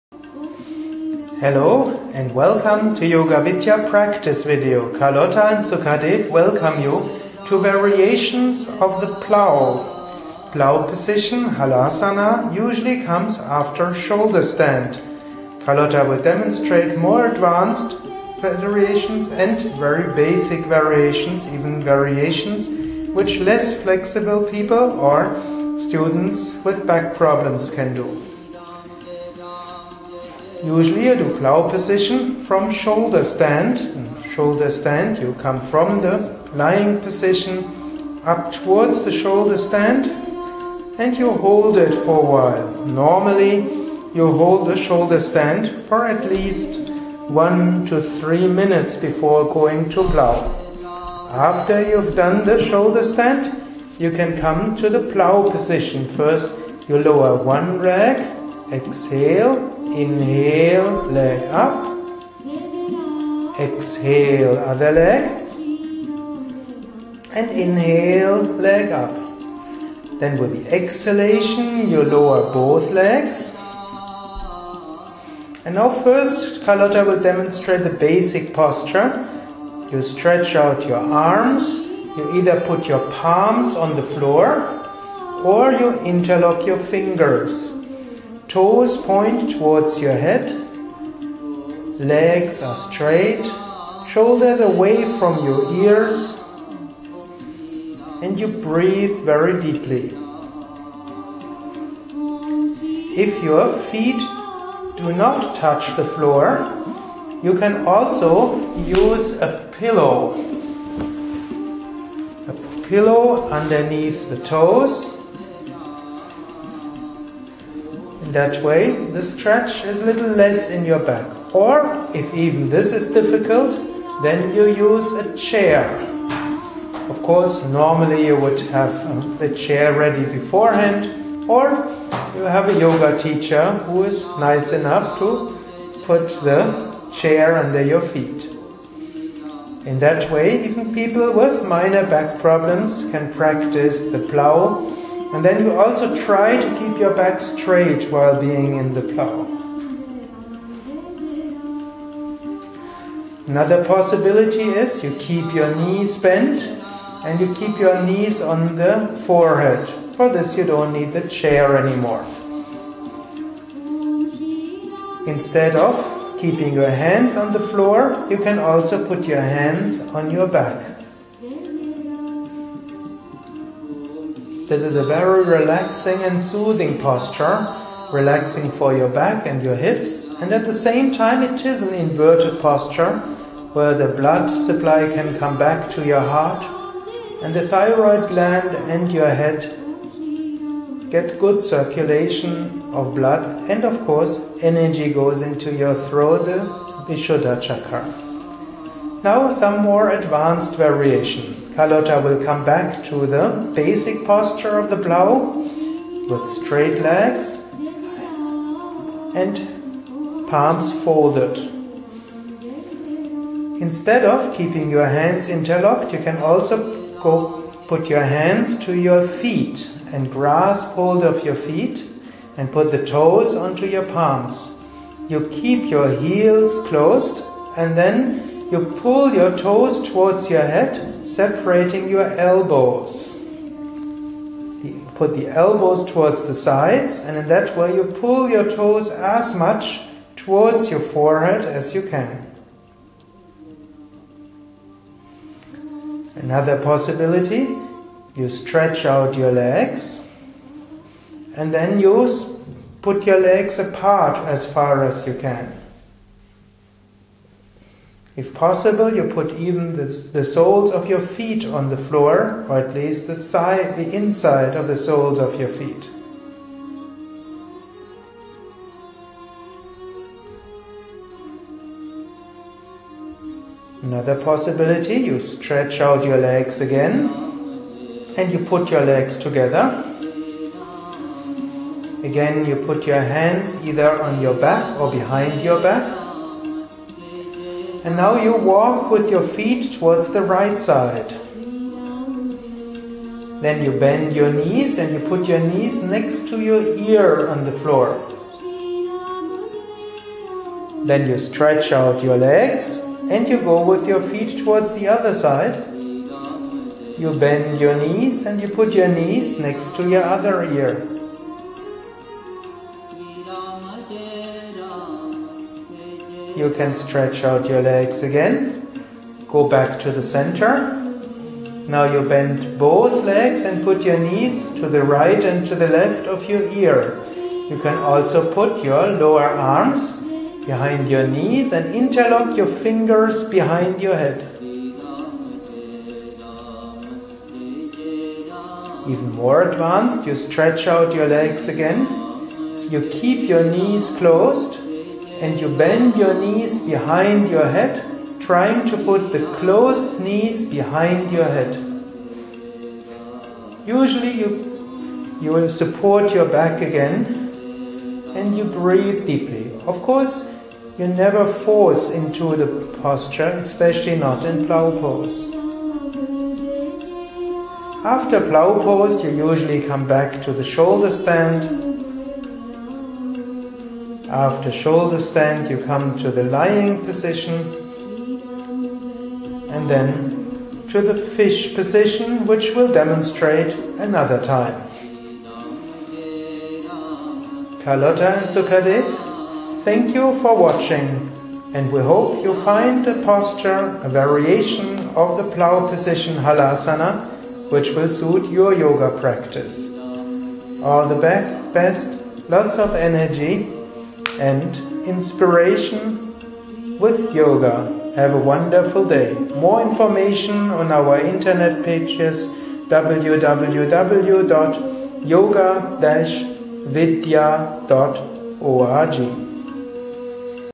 Yoga for Beginners and Advanced: Plough Variations
A "must" for yoga teachers and practitioners: Watch different variations of the hatha yoga exercise Halasana, the plough positions. Carlotta show you the basic posture, beginner's variations, intermediate variations and more advanced ones. Sukadev Bretz, founder of Yoga Vidya Germany, explains and guides you through this session.